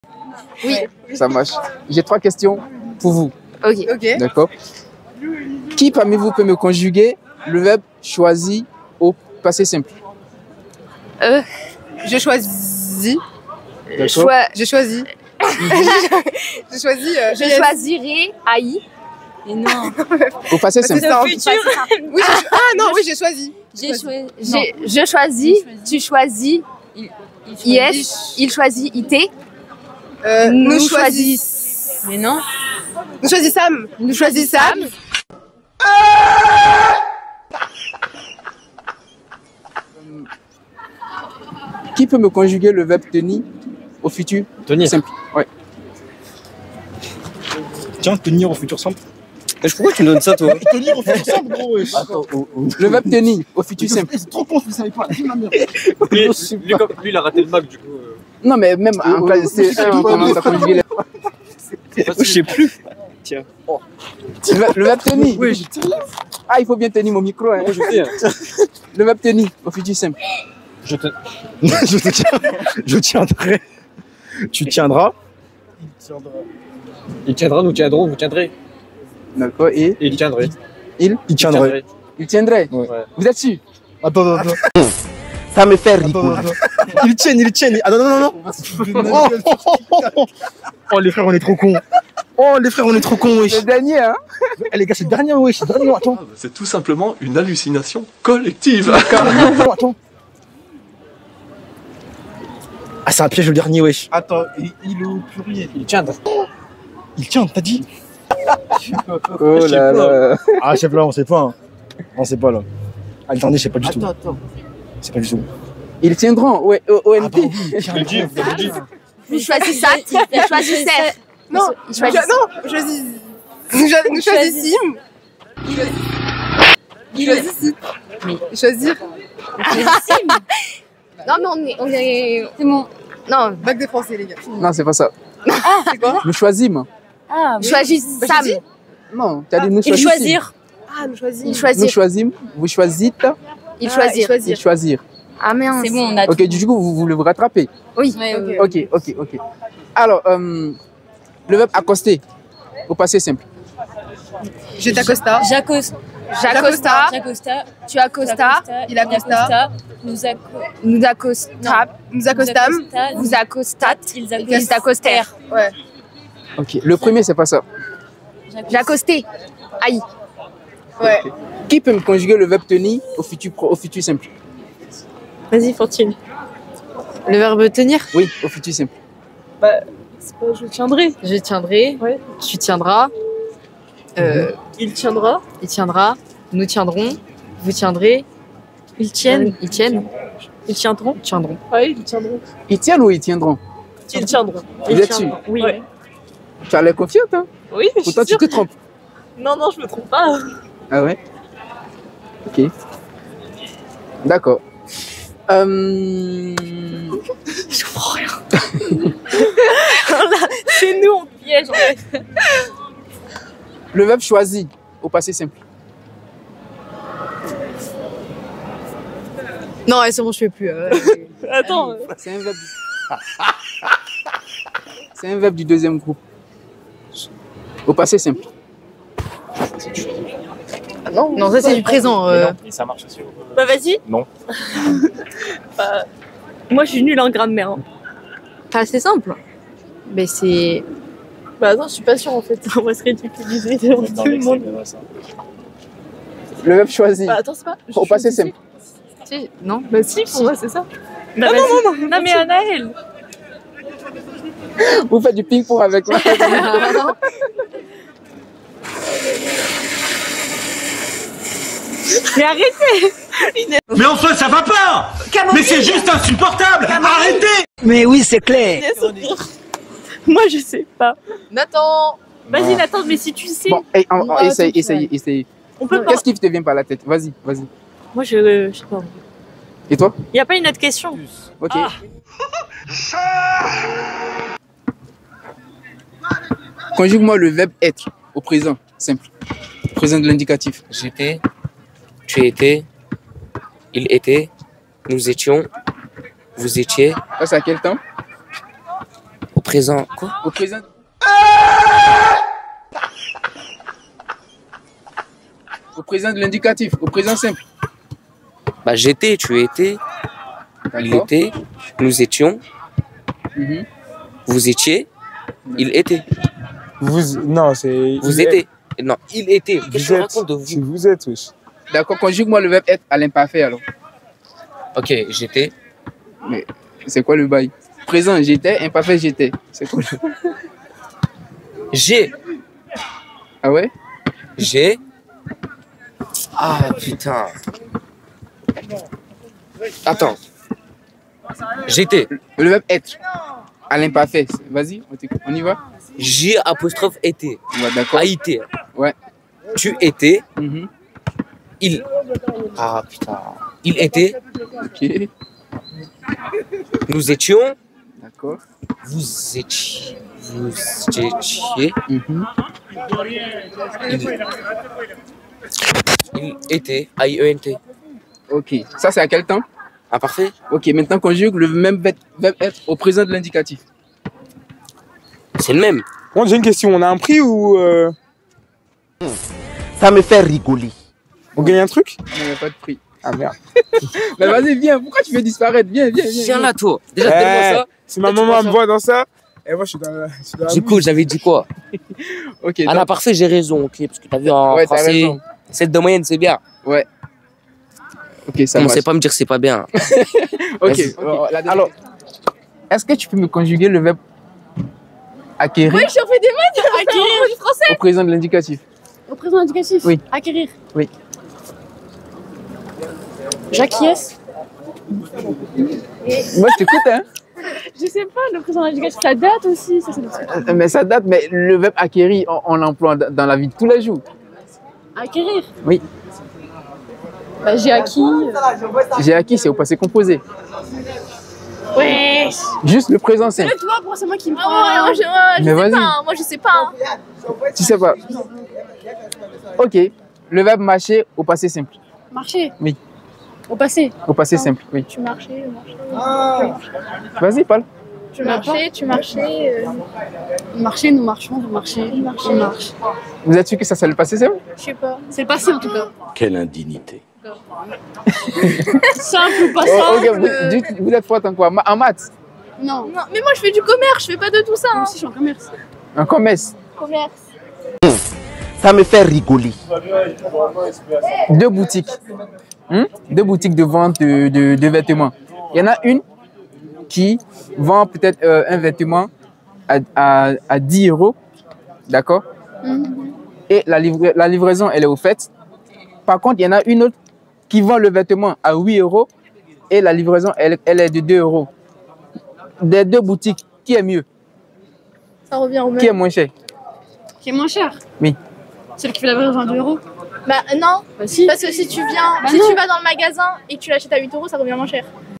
CONJUGAISONS
Aujourd'hui on fait de la conjugaison avec les gens interviewés